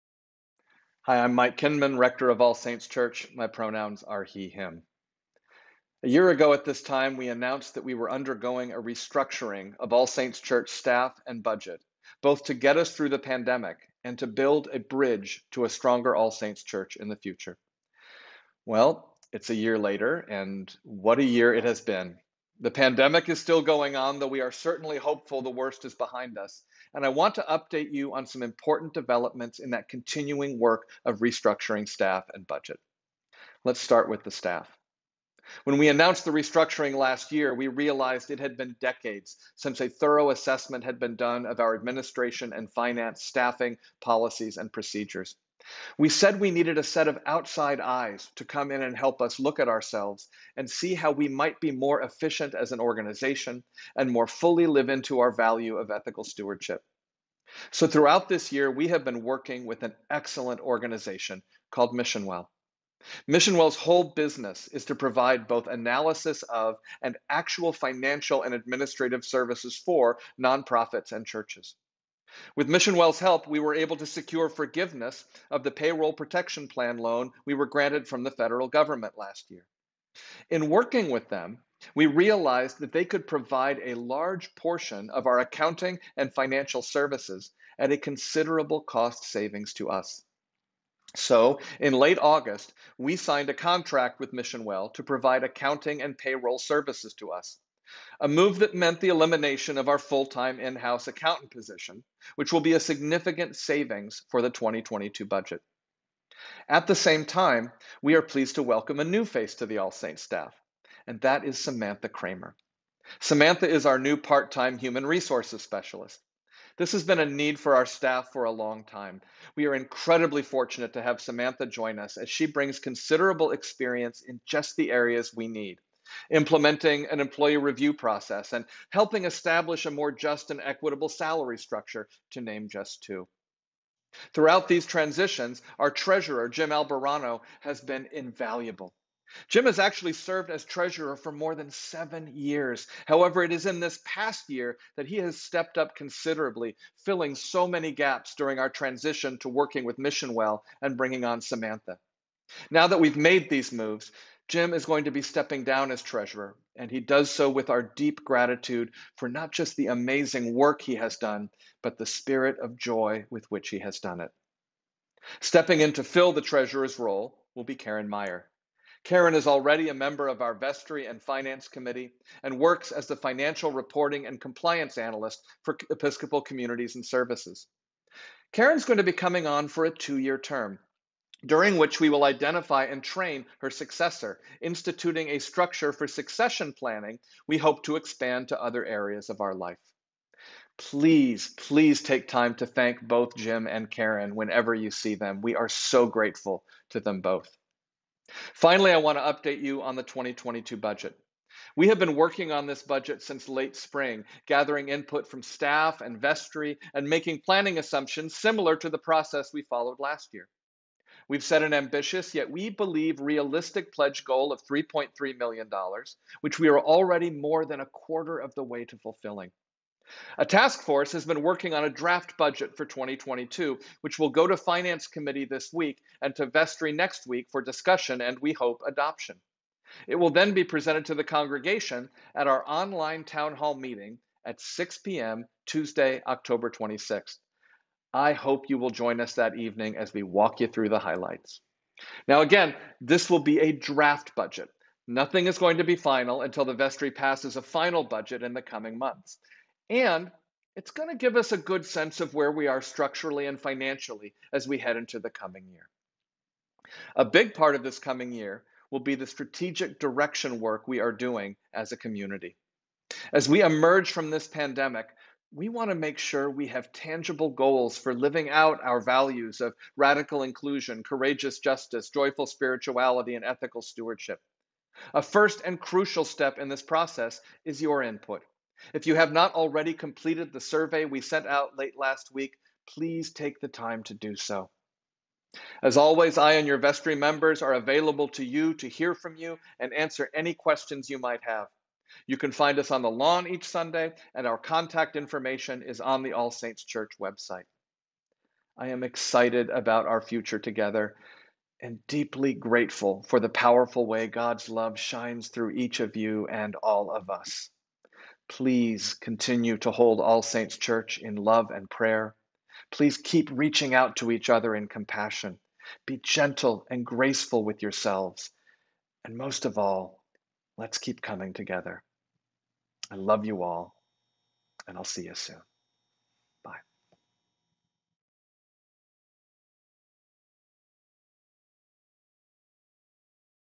All Saints Pasadena Financial Update from Rector Mike Kinman
Financial Update for All Saints Church, Pasadena, from Rector Mike Kinman on October 14, 2021.

Follow All Saints Church on Twitter @ASCpas.